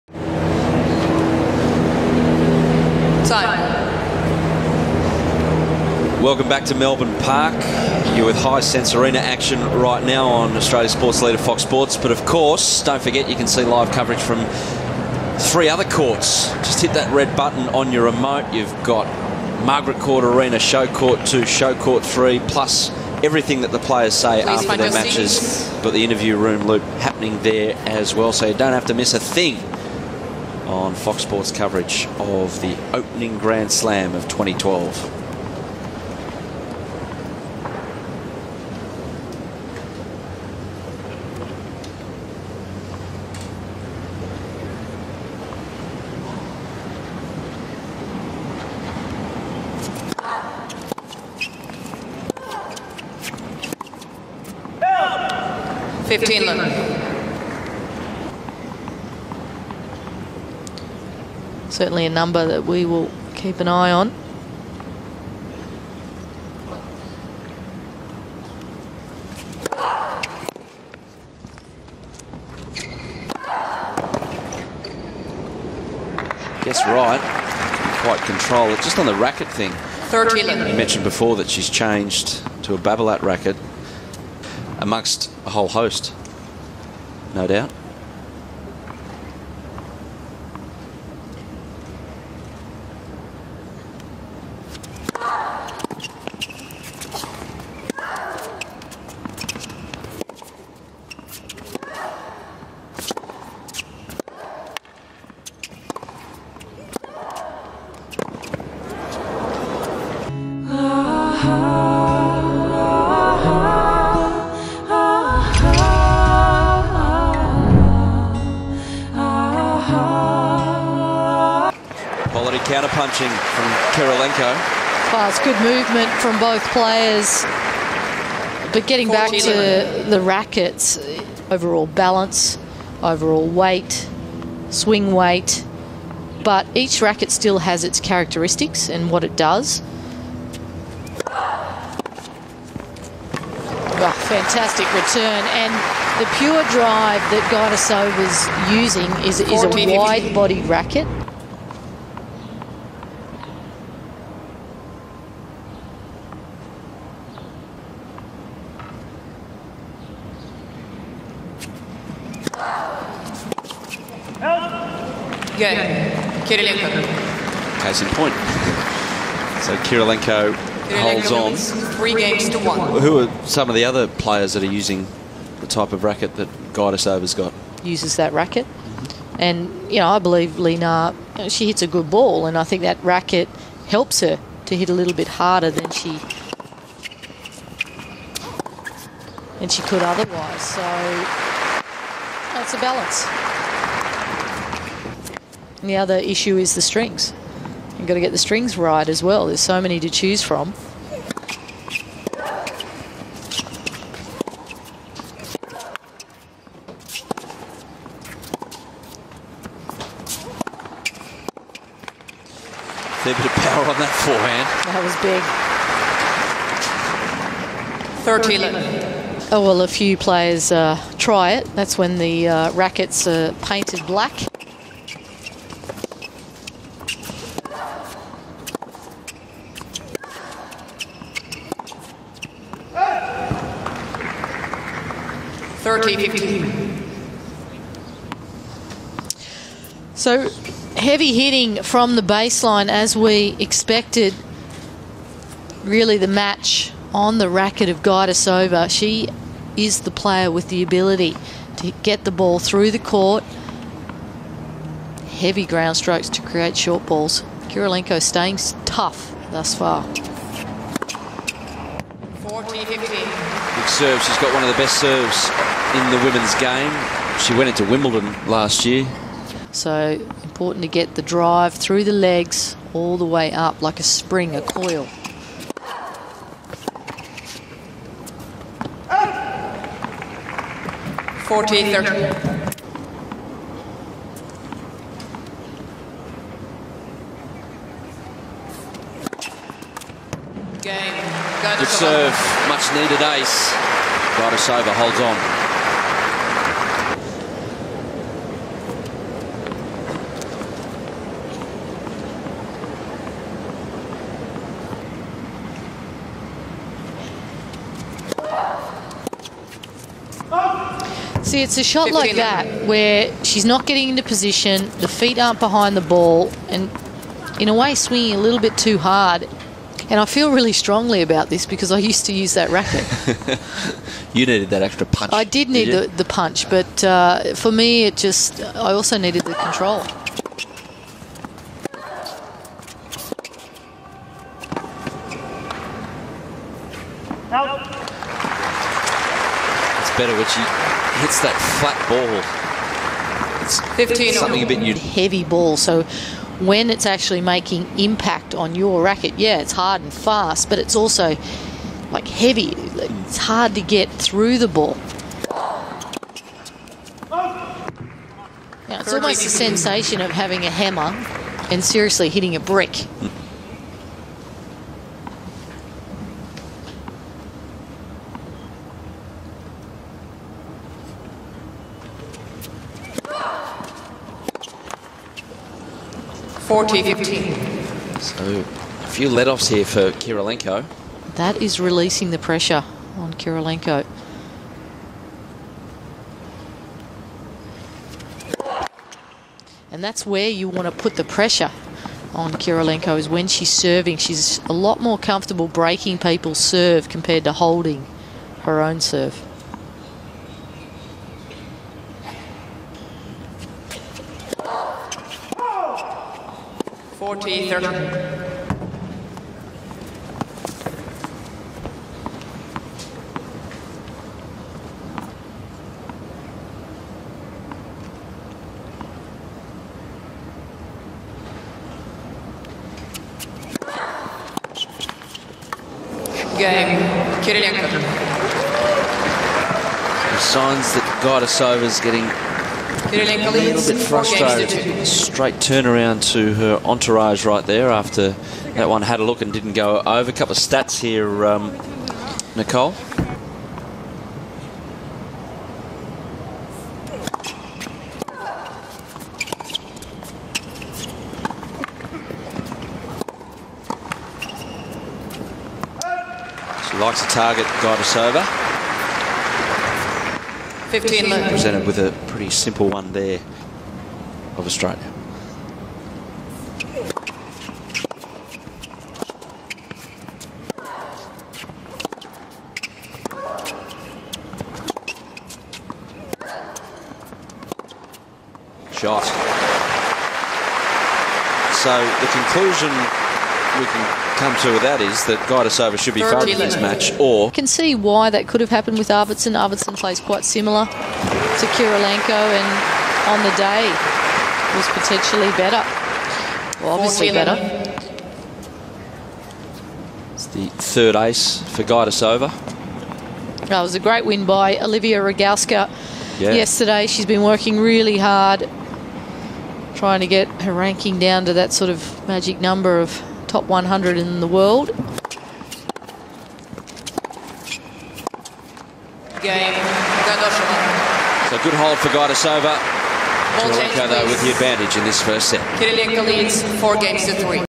Maria Kirilenko vs. Jarmila Gajdosova - AO 2012 (Part 2 of 6)
The Australian Open loves super-slow-motion replays... I edited these into the plays themselves along with some music and/or sound effects. I think it helps add to and accentuate the awesomeness that is Makiri.